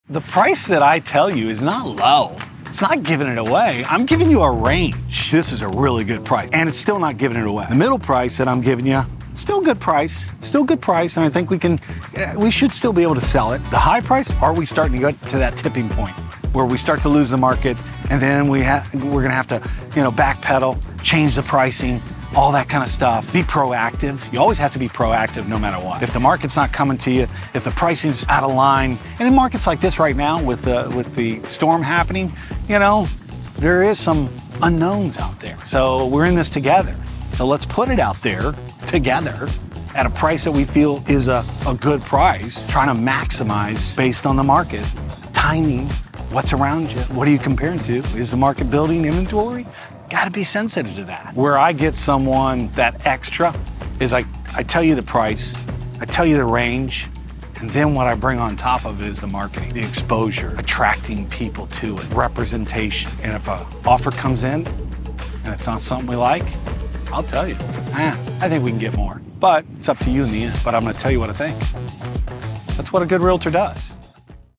𝐇𝐨𝐰 𝐭𝐨 𝐏𝐫𝐢𝐜𝐞 𝐚 𝐇𝐨𝐦𝐞 💰 SWFL Real Estate Selling Tips
When you work with McMurray & Members as your SWFLrealtor, you can count on us to price your home realistically and competitively based on the current market.

We’ll give you a price range with three tiers of prices (low, middle and high) so you can understand where your listing fits in the SWFLrealestate landscape.

Here are 3 things we consider when pricing your home:

🔹 Is the market building inventory?
🔹 How much demand is there in the marketplace?
🔹 What are comparable homes selling for right now?

Our listing prices are always based on the state of the market so they are most likely to move.

Once your listing goes live on the MLS, we maximize exposure by continually marketing it to qualified leads. Our approach is strategic, systematic and proven to work.

Are you ready to be represented by the best?